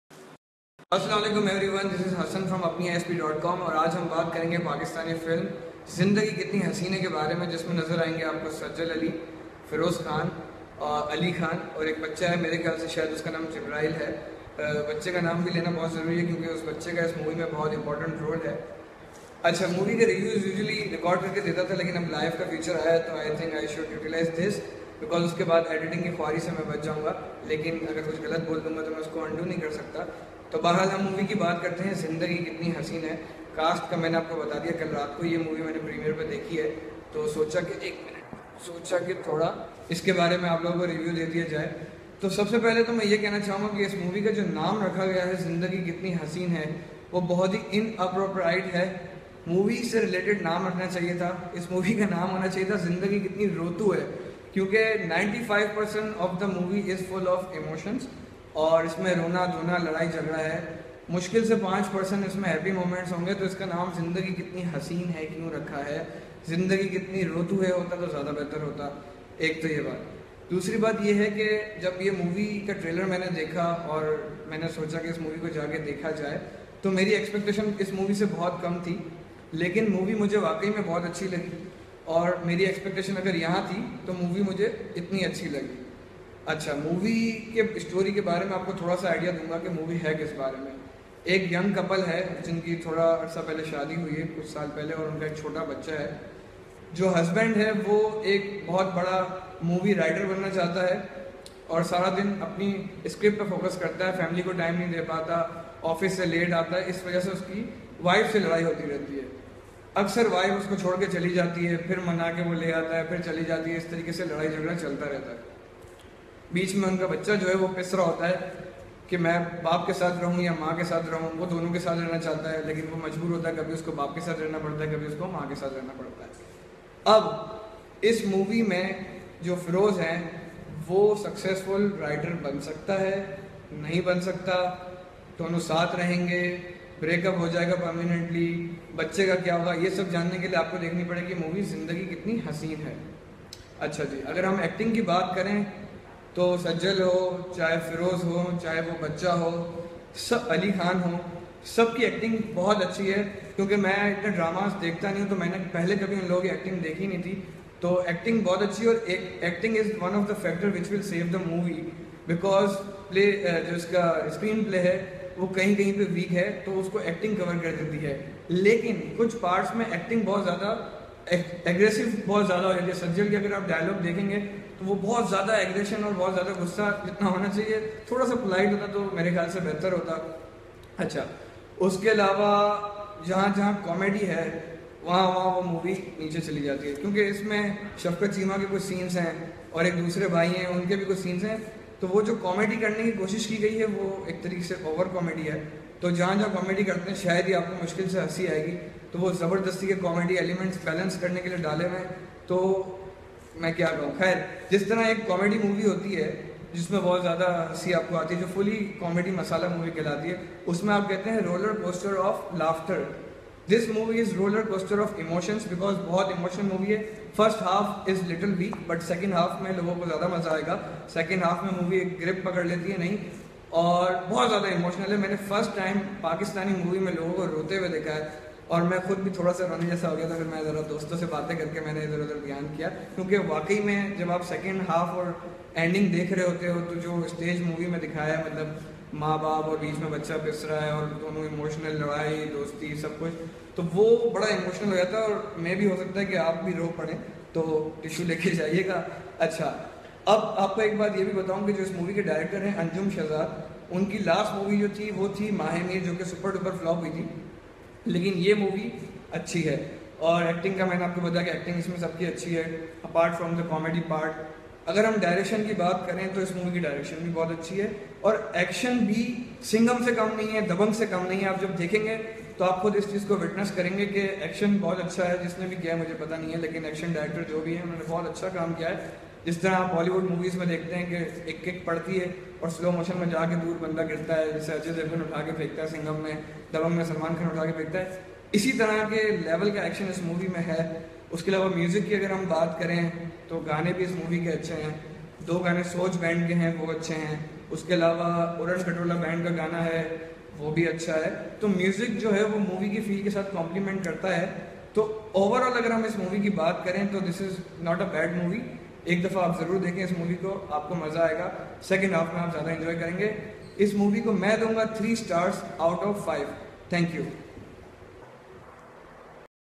Zindagi Kitni Haseen Hai (Review)
Review of upcoming Pakistani film Zindagi Kitni Haseen Hai starring Sajal Aly & Feroze Khan by Hassan Ali from ApniISP.Com